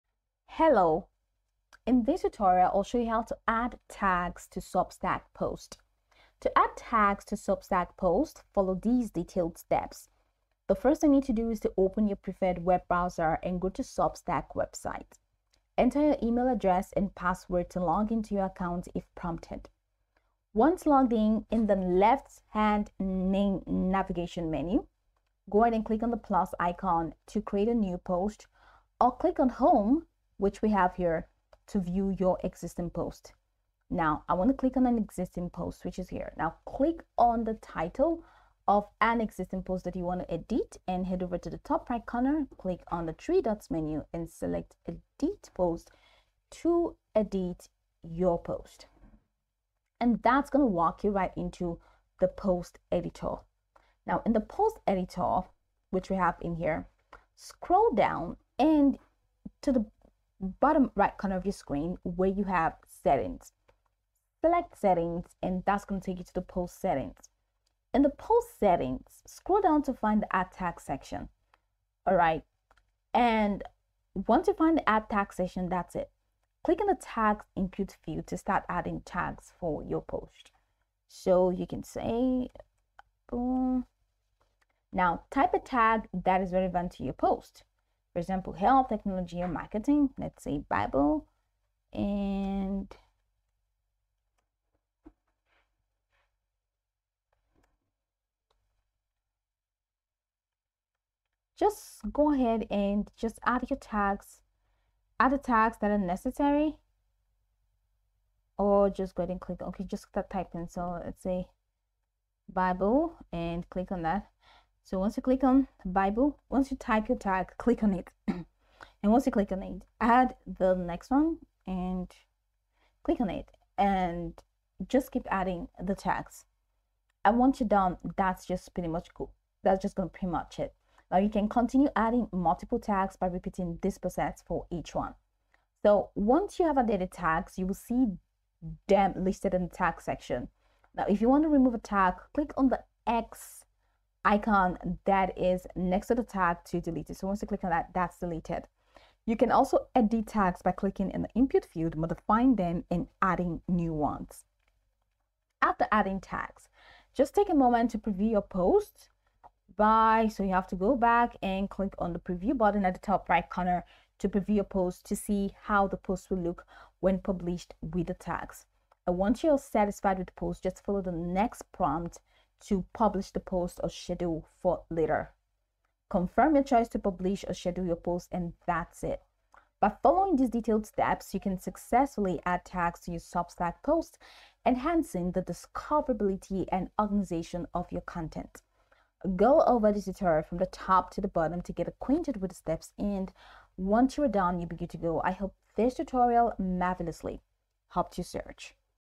How to Add Tags to Substack Posts (Quick Tutorial)
In this video I will show you How to Add Tags to Substack Posts. It would be good if you watch the video until the end so that you don't miss important steps.